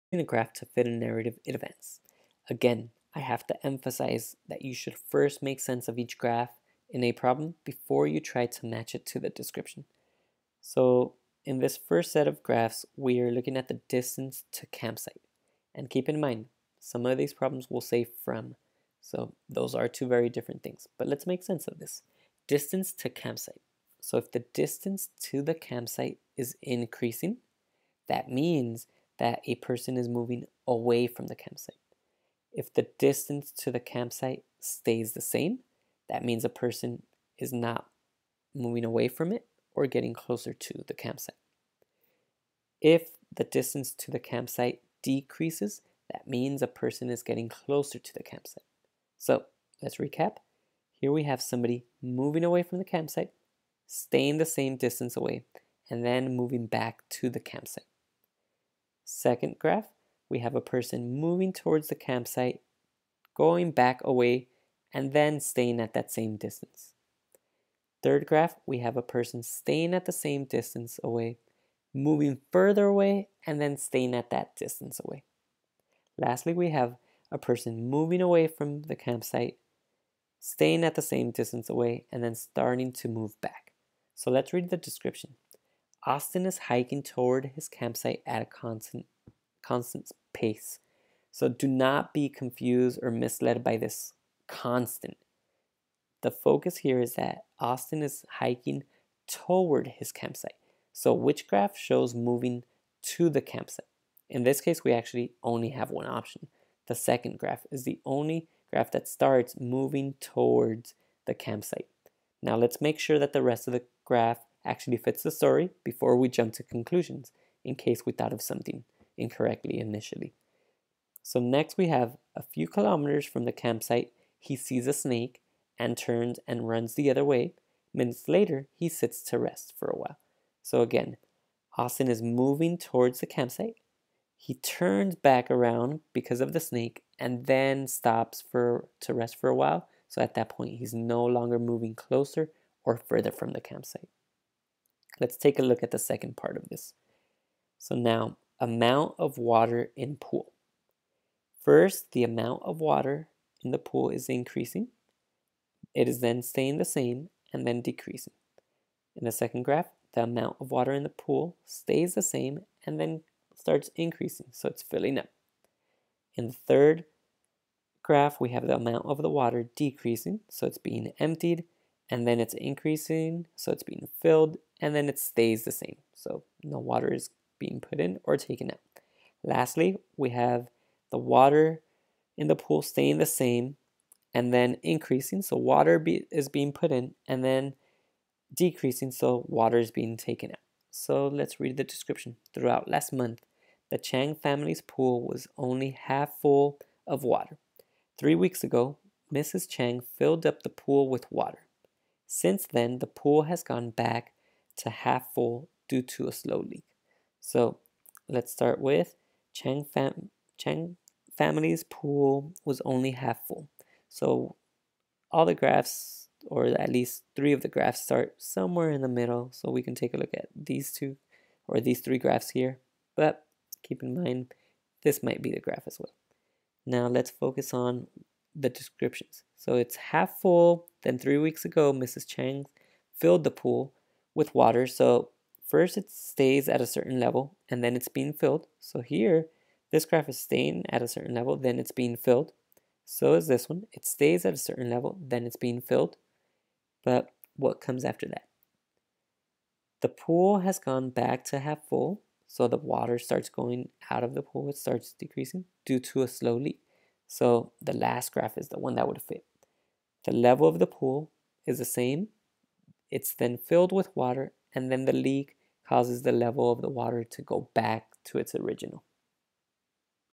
10a Type 6 Narrative Advanced#21-24
This video lesson will teach you how to choose a graph to fit a narrative (advanced).